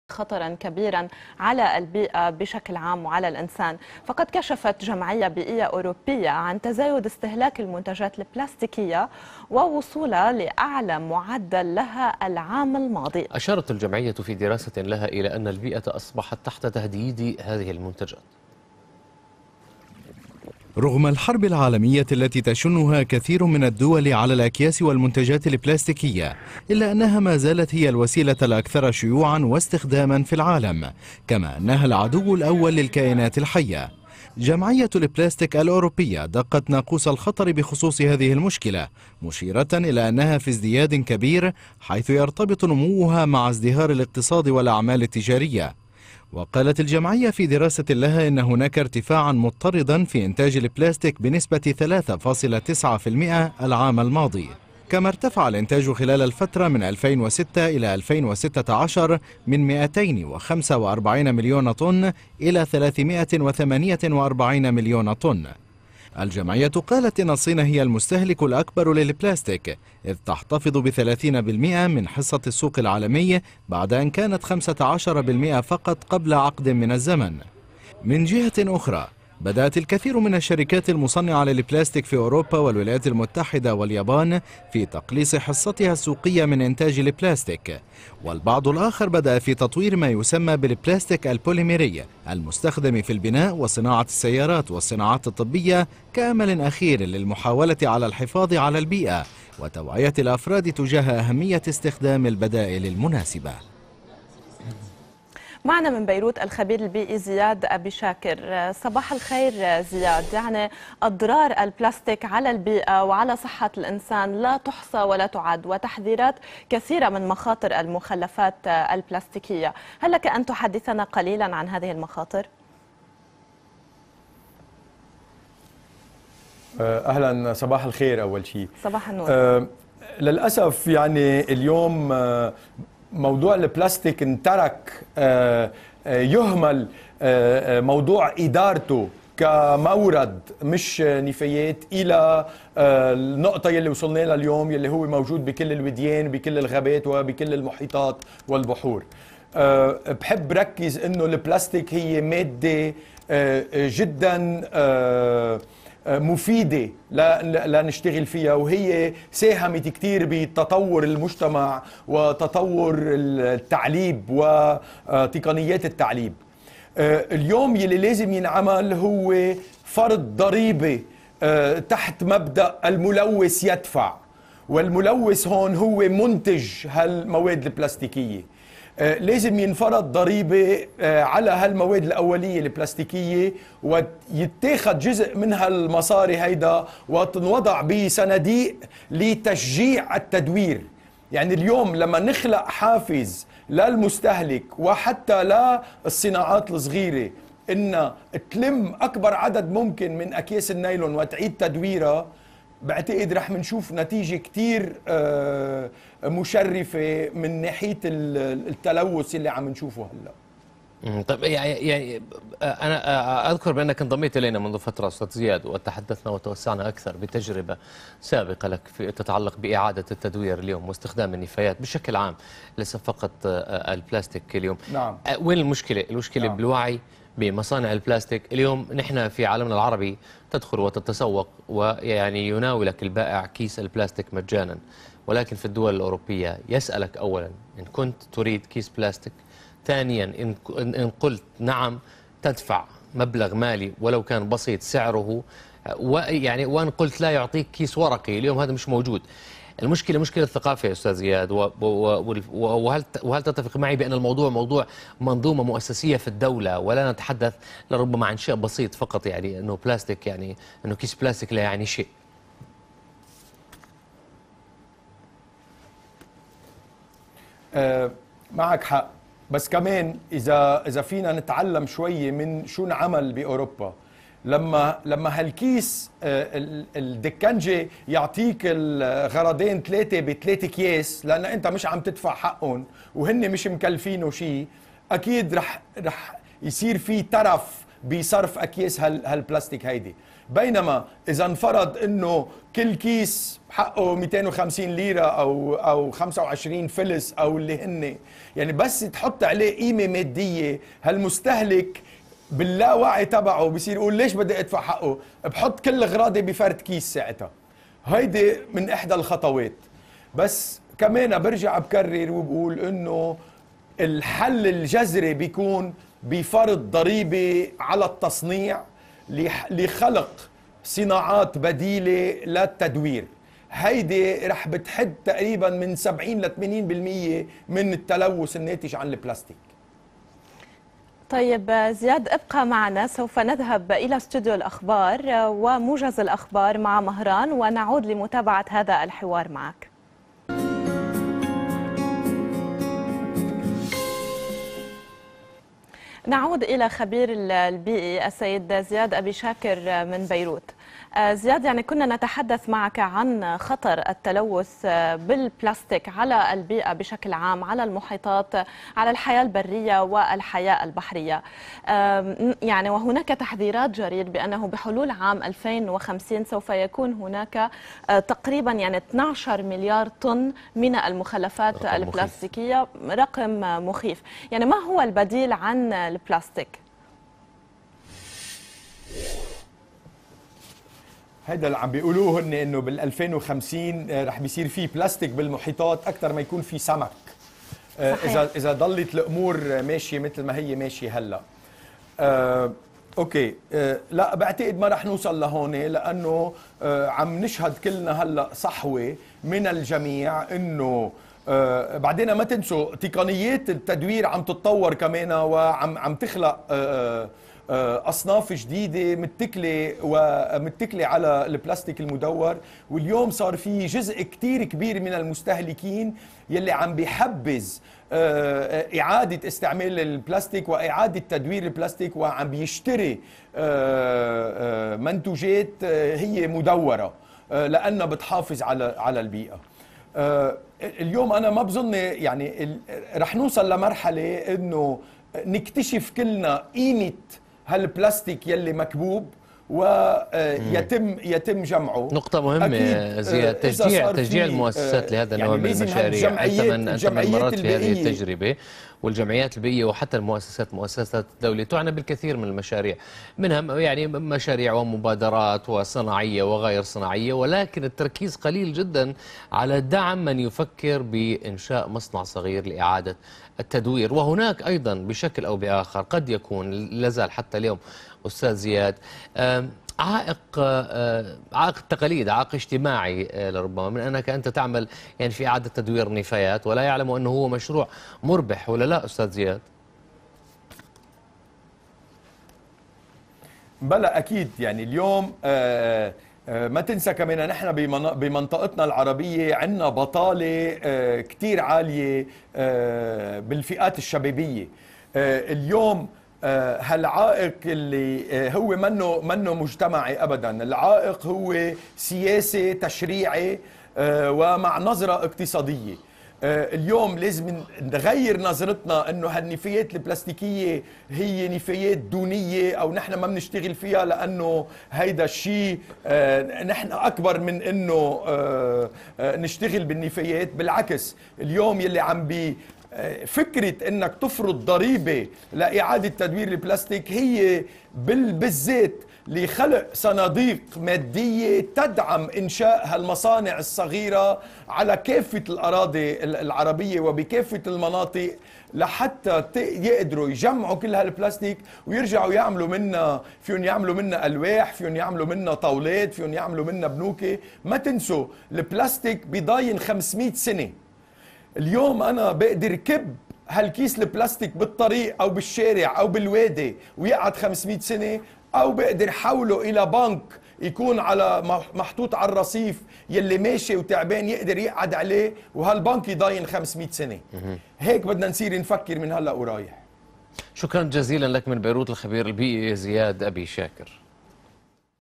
Sky News July 2018
Interview with Ziad Abichaker about the rampant plastic pollution and the solutions proposed to curb this problem... Sky News Arabia July 9 2018